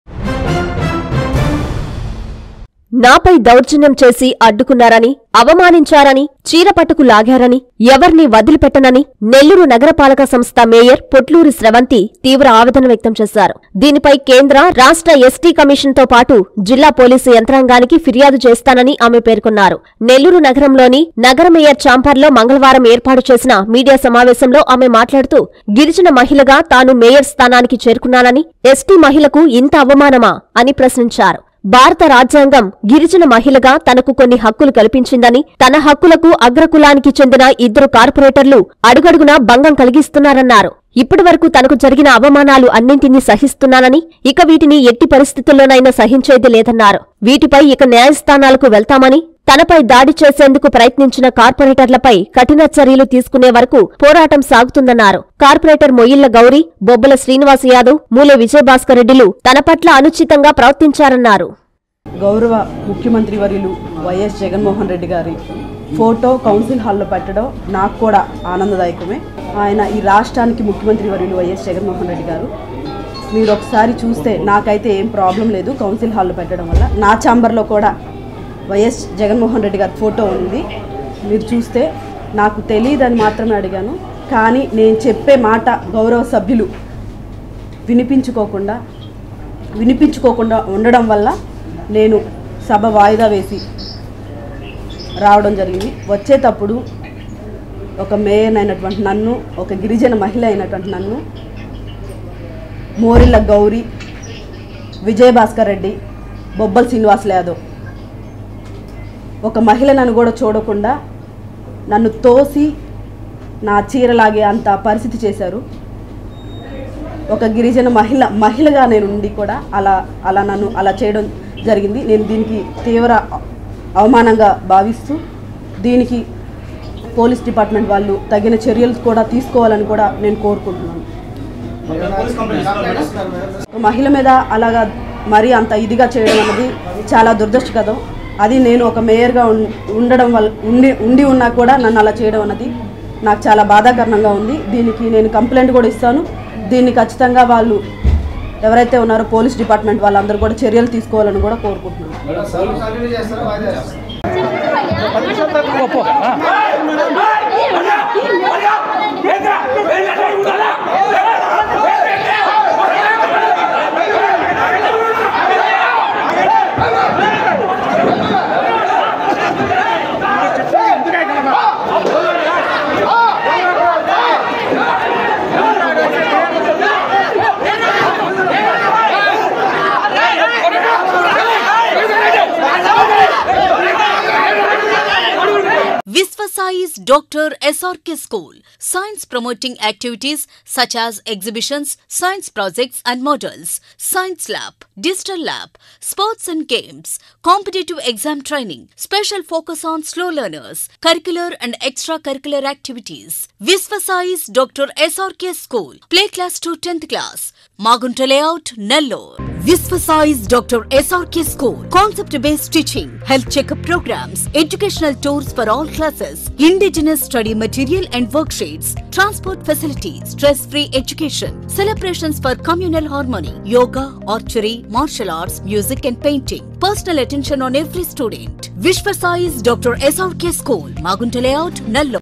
నాపై దౌర్జ‌న్యం చేశారు...
- ఎస్టీ కమిషన్,  జిల్లా పోలీసు అధికారులకు ఫిర్యాదు చేస్తా
- నెల్లూరు కార్పొరేష‌న్ మేయ‌ర్ పోట్లూరి స్ర‌వంతి